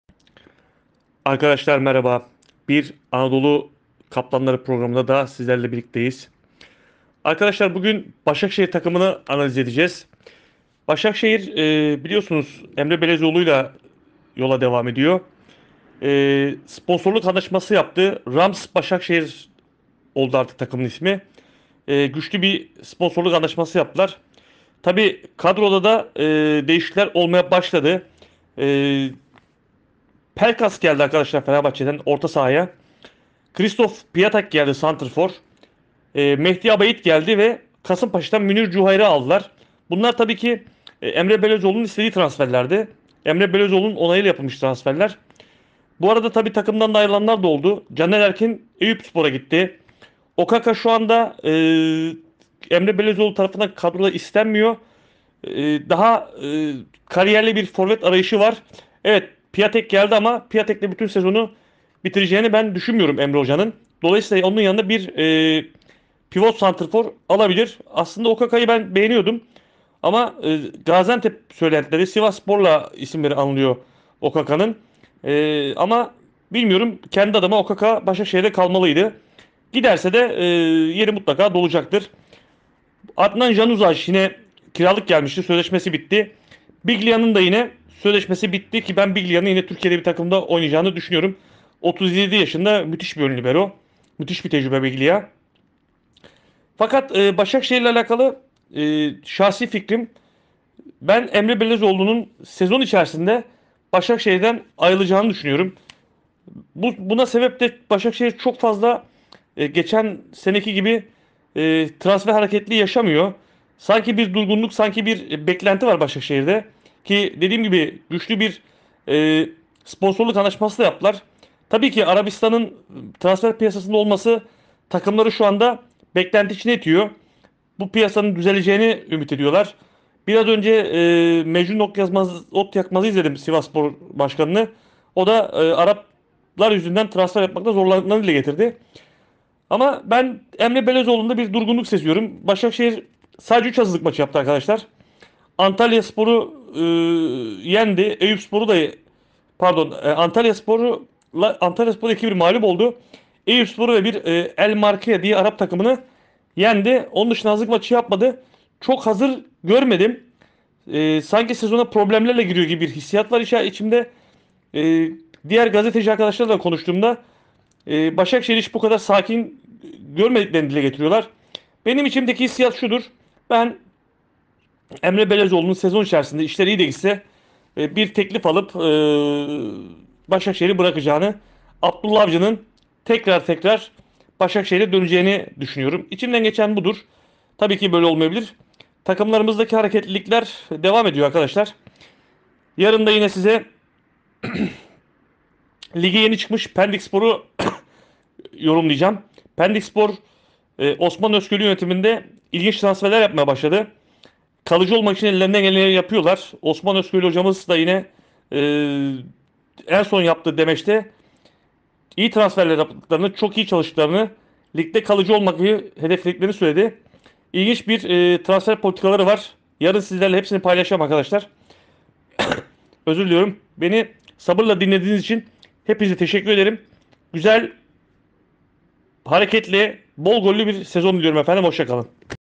Anadolu Kaplanları / İstanbul Başakşehir Futbol Kulubü
Başakşehir Futbol Kulubü'ne Dair Tüm Olan Bitenleri B@cobetting Editörü Ercan Tekin Sizler İçin Değerlendiriyor ... Keyifli Seyirler...